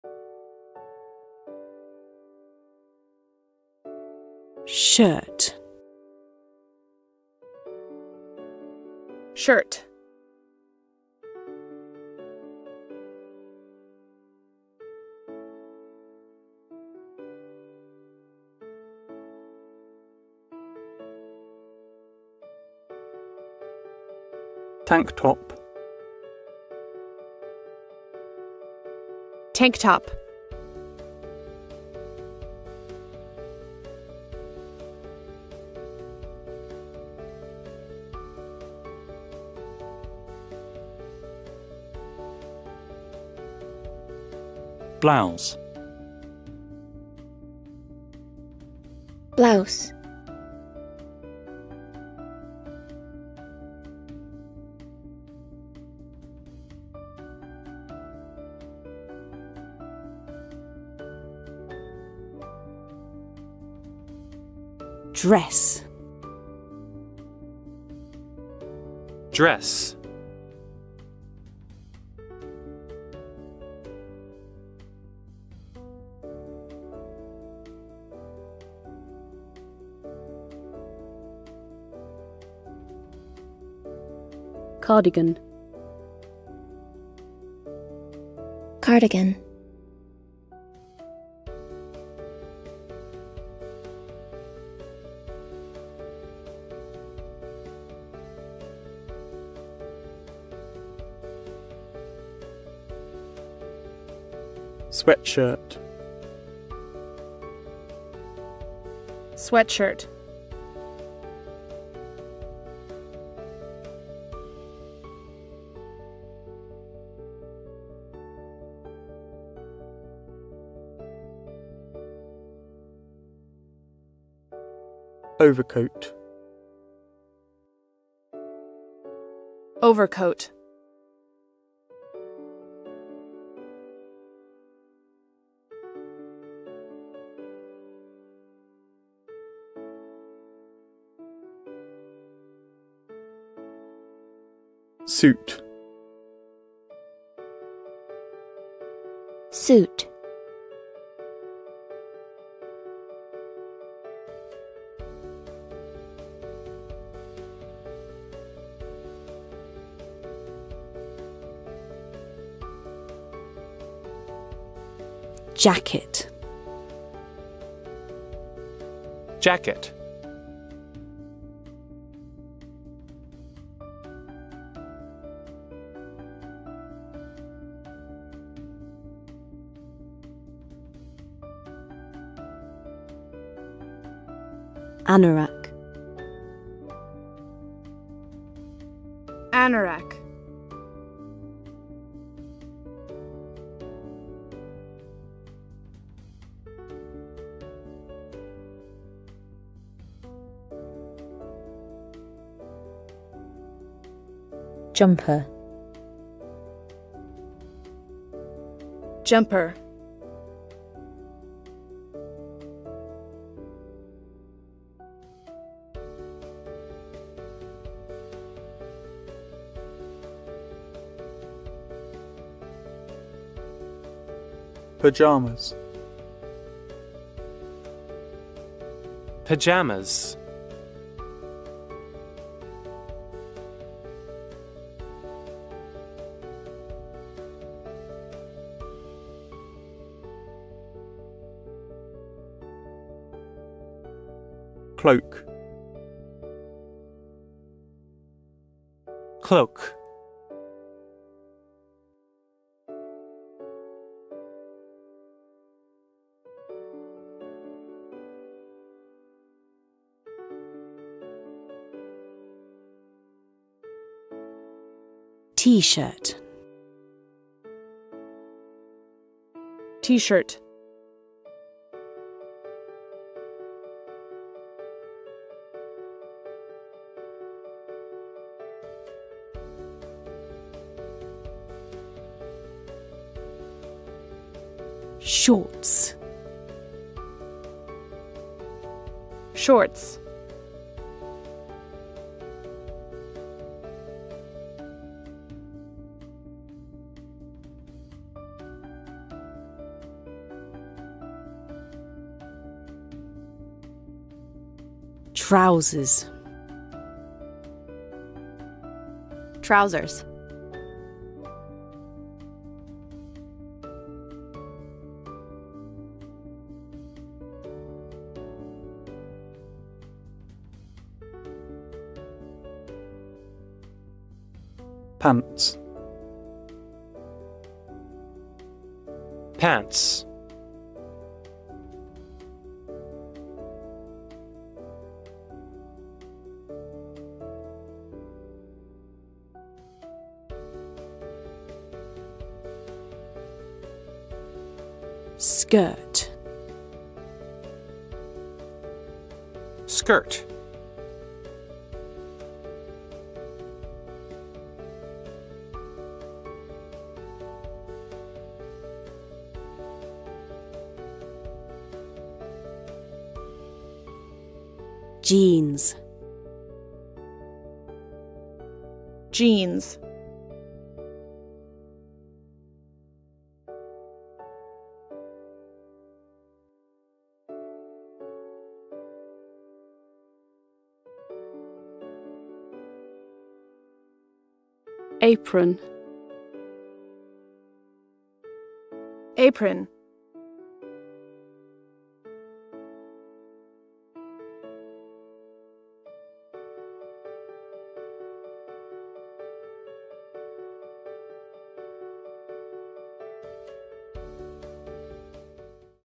Clothes and Fashion (Part 1)
Trong video có 2 kiểu giọng United Kingdom và United States.
Kiểu giọng UK được phát trước, kiểu giọng US được phát sau.
Lời khuyên: Các bạn nghe và não bộ xử lý âm thanh, gán âm thanh vào hình ảnh thấy được. Sau đó lặp lại âm thanh và học thêm mặt chữ sau cùng.